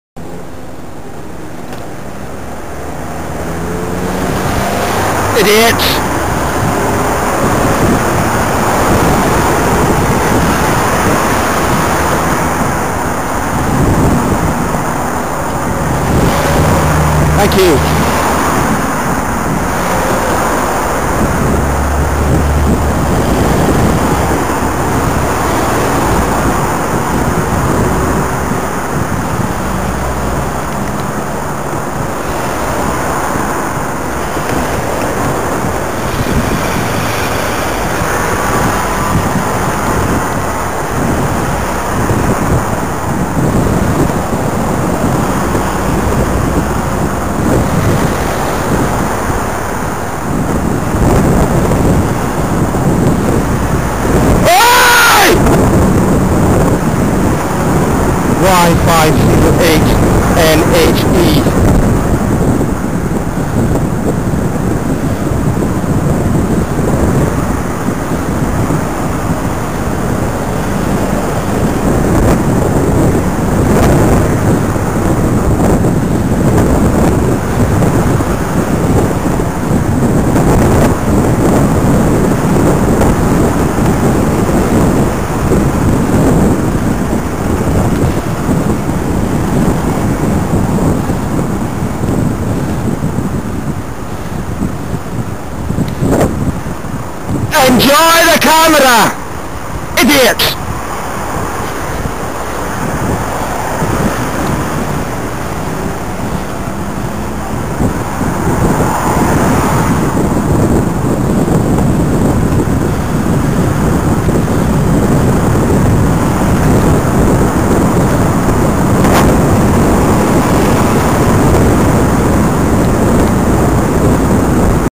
Y508NHE - as promised your few seconds of idiocy, on film
You had kids in the car - and I'm guessing you don't allow them to cycle much, 'cos the roads are too dangerous.  News for you, "driver".

The roads are only dangerous because of ignorant, complacent fools who squeeze past cyclists, at speed, with less than a foot to spare .like YOU.

The other drivers passed at least adequately - or even excellently.  But yours?  And totally unnecessary - I sail past a few seconds later at the lights.